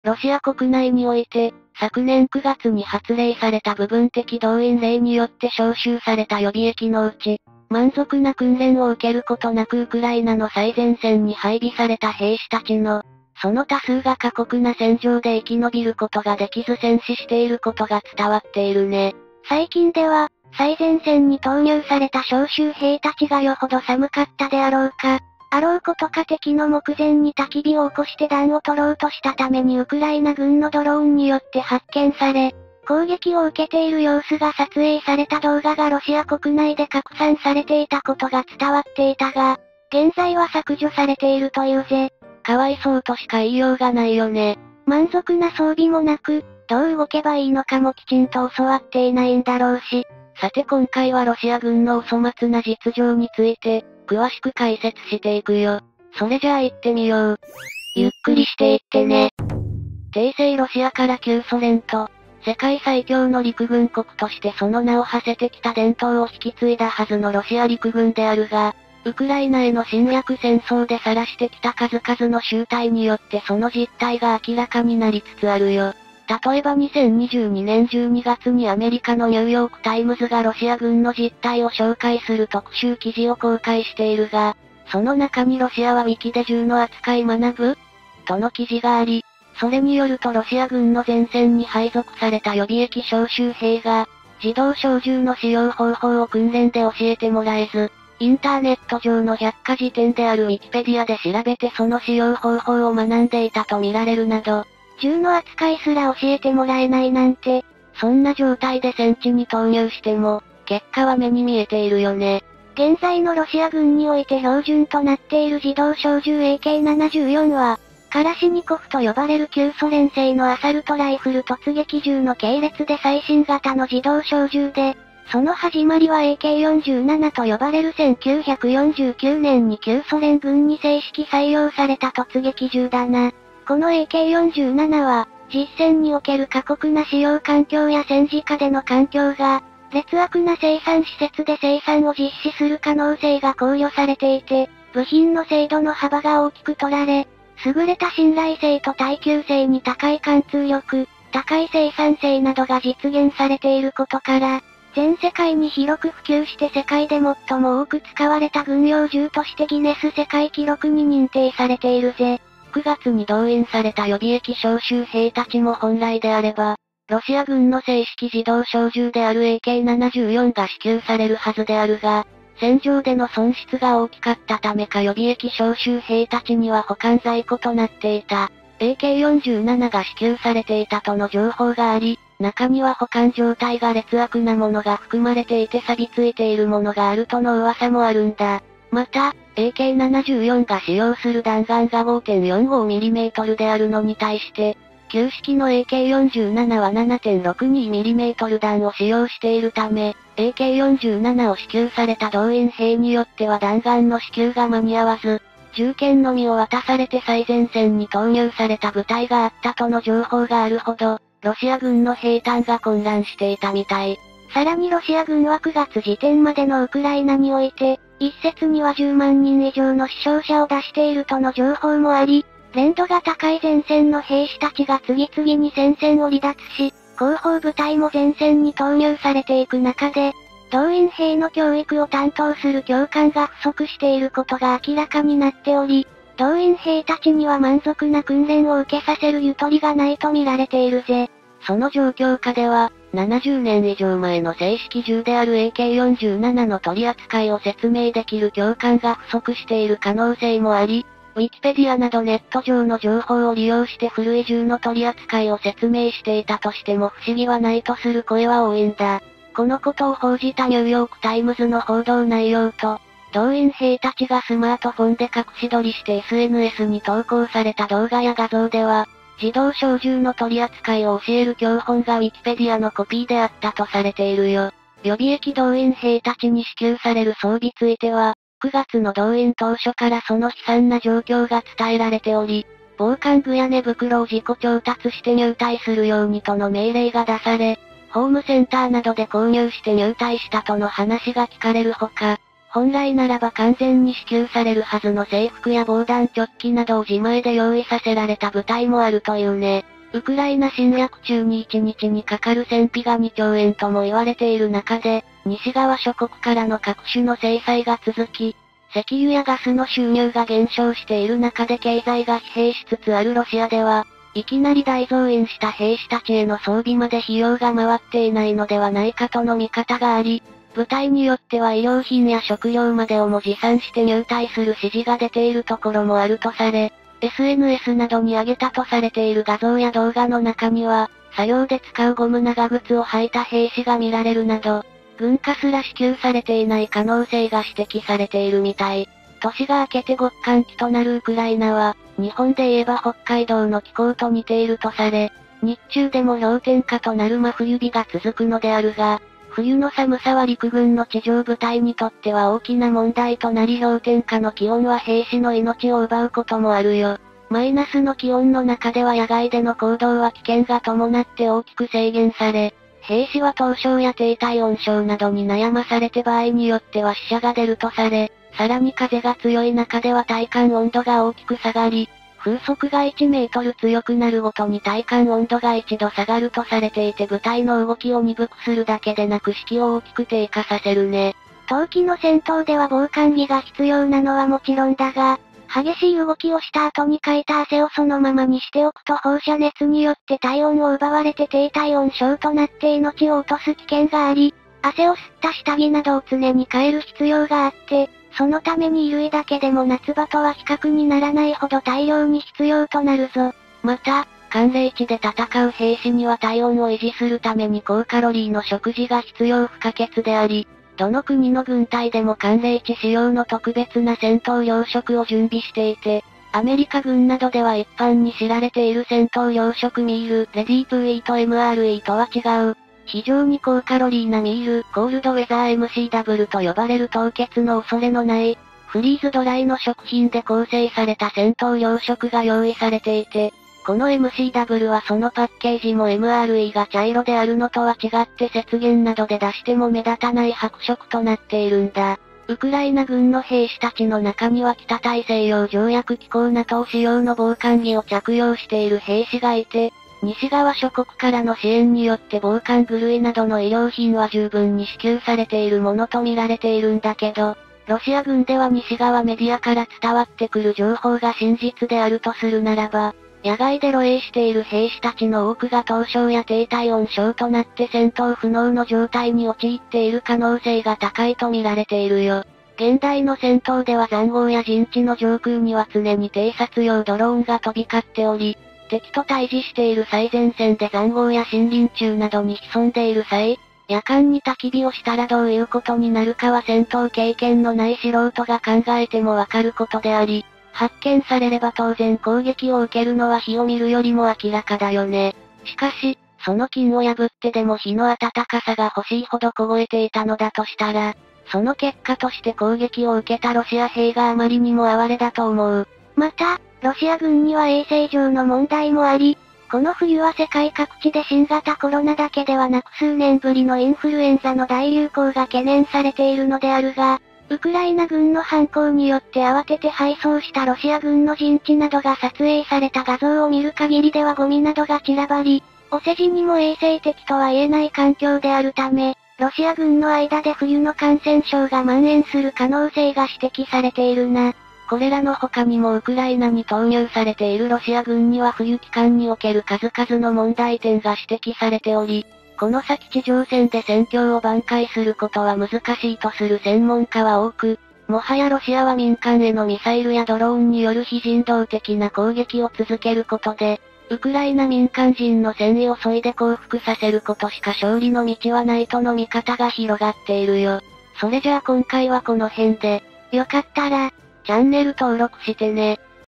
【ゆっくり解説・軍事News】ウ東部クレミンナ奪還撤退開始ロ軍動員兵による焚き火でウ軍発見kか！ドローン攻撃で露に動画拡散・・・
引用・転載「出典：海上自衛隊ホームページ」
引用・転載「出典：航空自衛隊ホームページ」
引用・転載「出典：陸上自衛隊ホームページ」